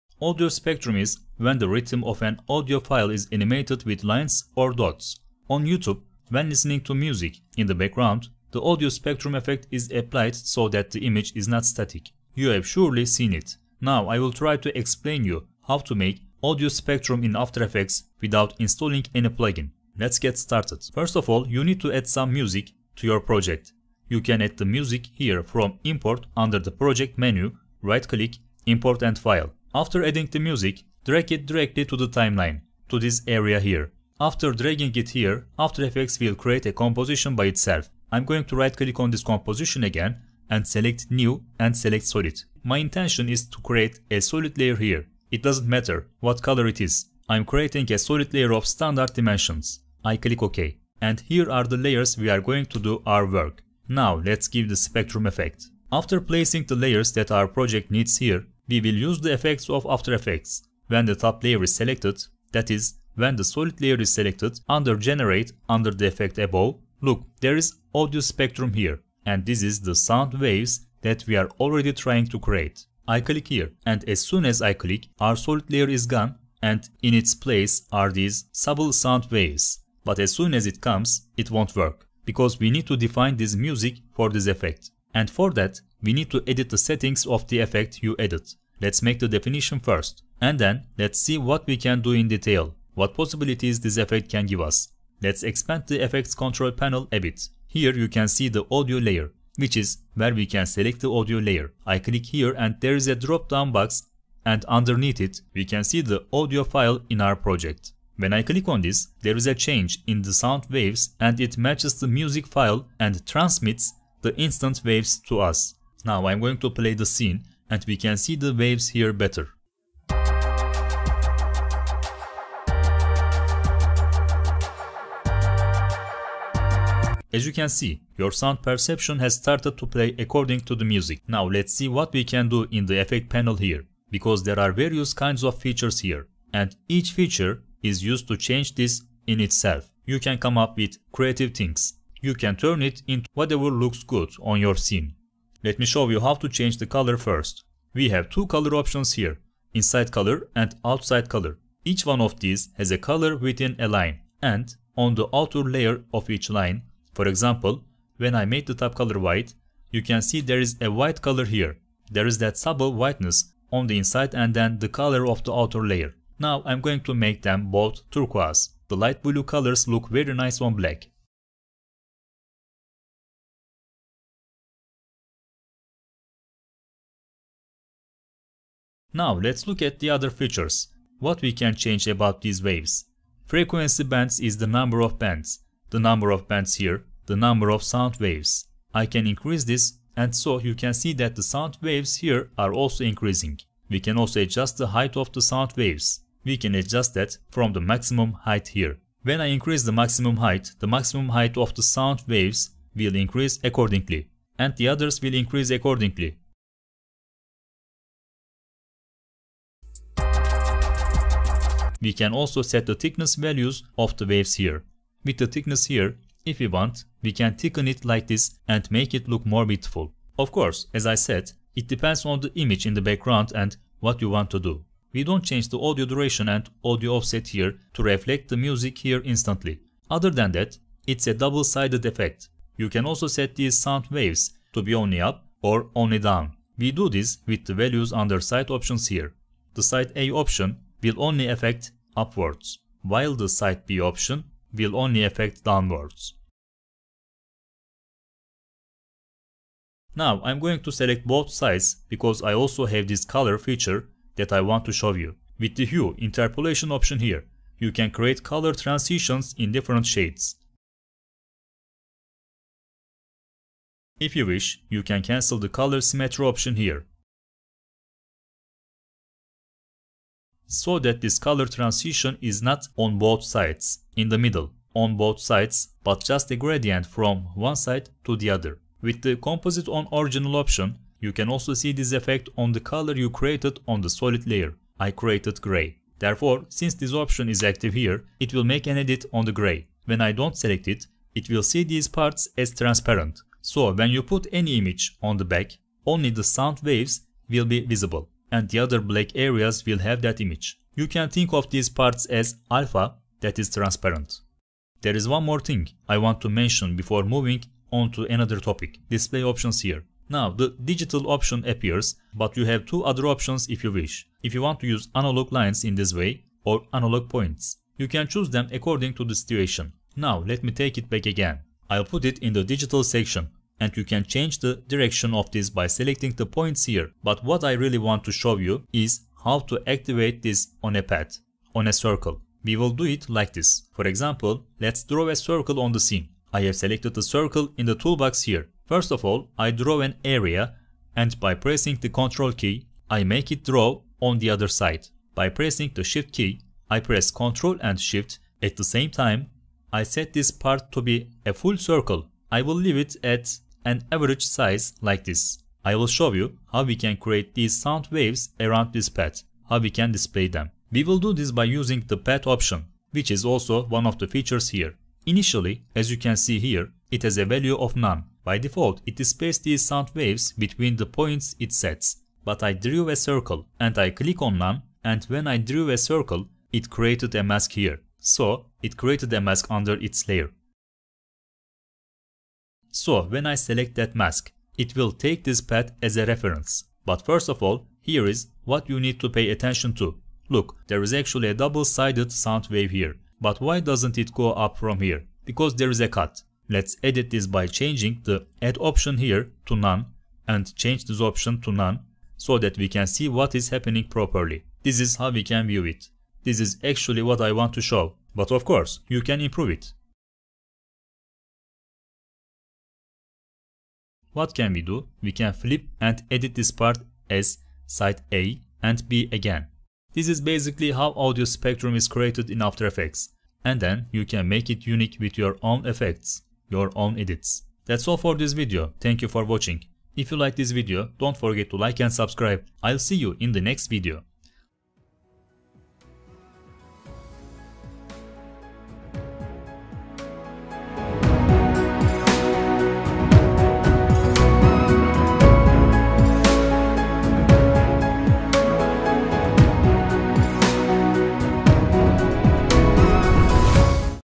After Effects AUDIO SPECTRUM: Audio Visualizer on Circle Path (No Plugin)
Audio Spectrum is the rhythm of an audio file animated with lines or dots. How to make an Audio Spectrum sound effect / audio visualizer on circle path in After Effects without any plugin?